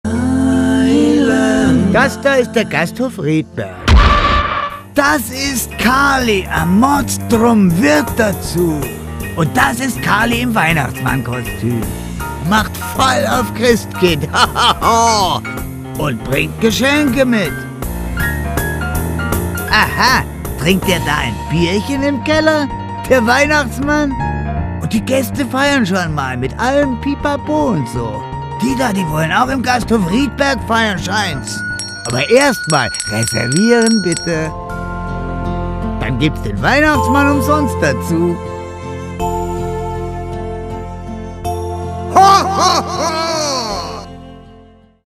BIERGASTHOF RIEDBERG | Weihnachten feiern
Reservieren Sie schon jetzt Ihre Weihnachtsfeier! Darauf ein Bier!

Firmen, Vereine, Stammtische und sonstige Gemeinschaftsaktivitäten. Feiern Sie Weihnachten im gediegenen Rahmen, begleitet von Bier und Bierspezialitäten. Schon reserviert? Es ist nie zu früh! Darauf ein Bier!